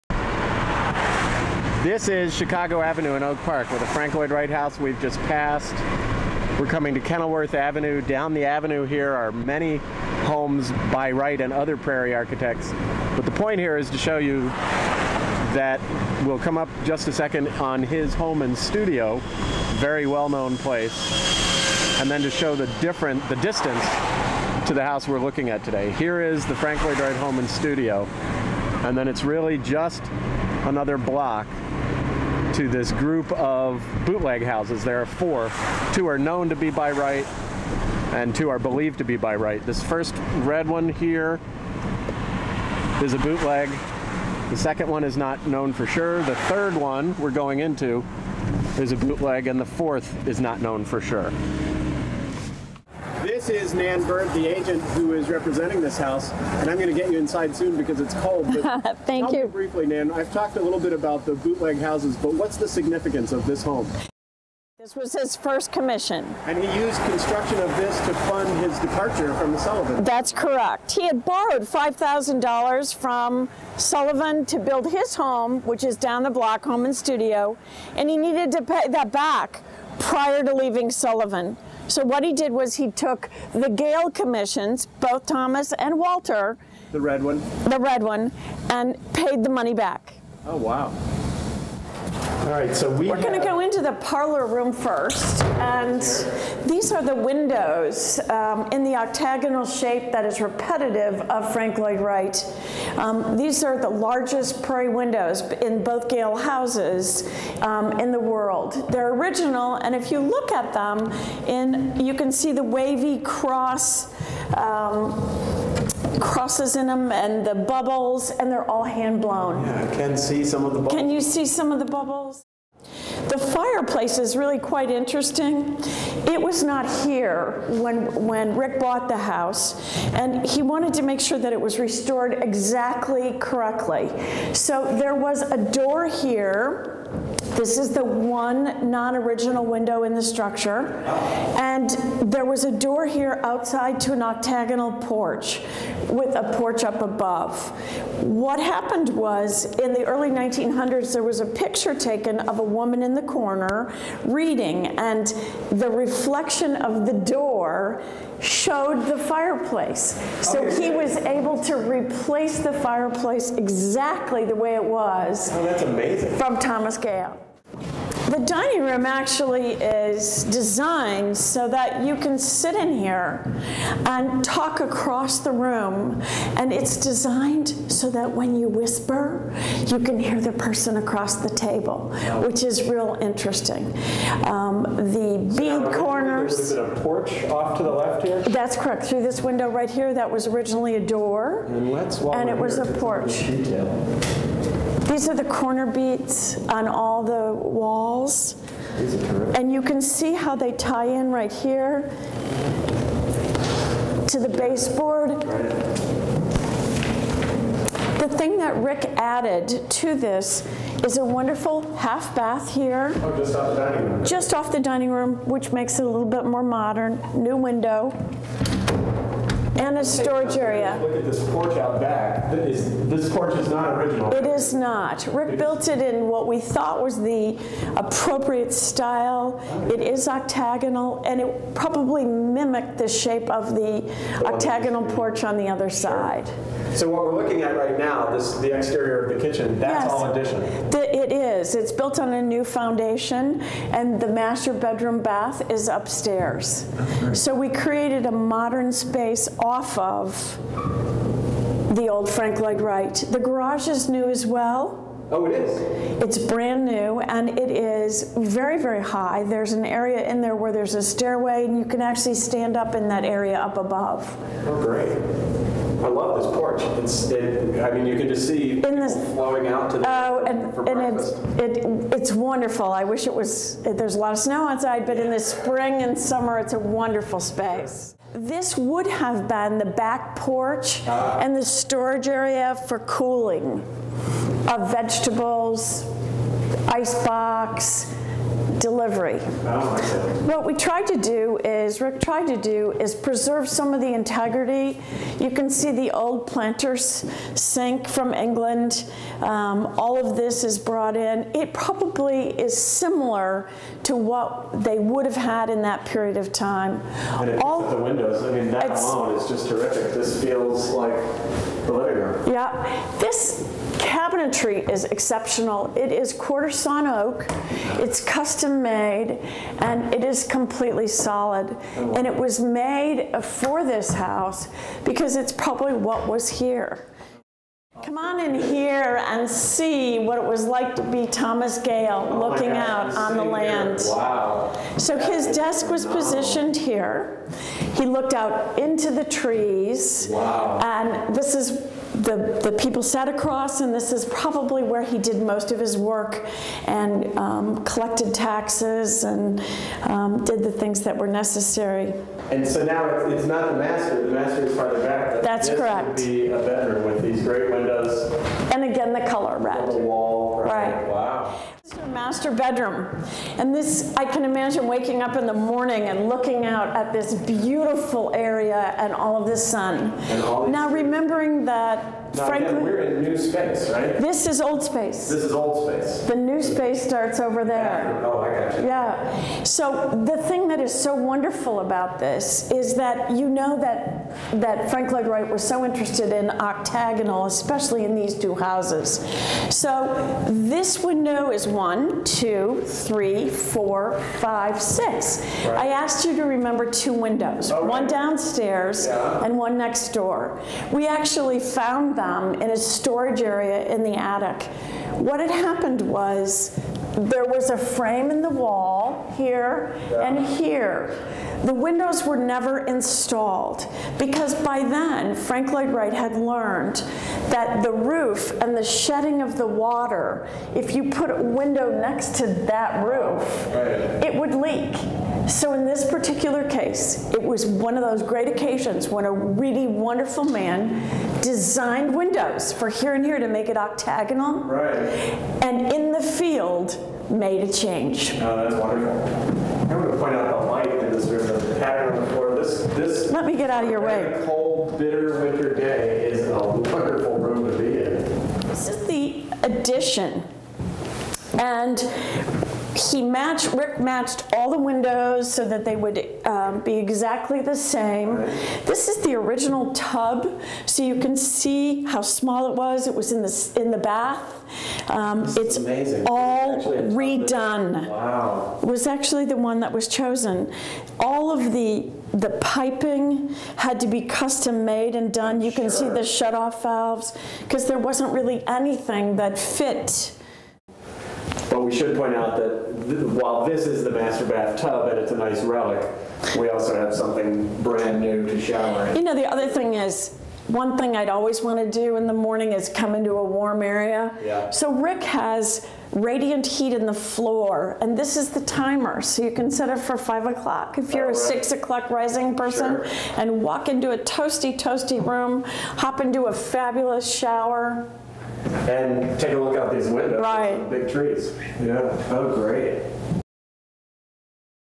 Deal Estate: Wright Redone Right in Oak Park
List Price: $1.295 million
The Property: This house, one of a trio that Frank Lloyd Wright designed in his off-hours while employed by Louis Sullivan in the early 1890s, has been extensively renovated and expanded, all with a careful eye on the home's history...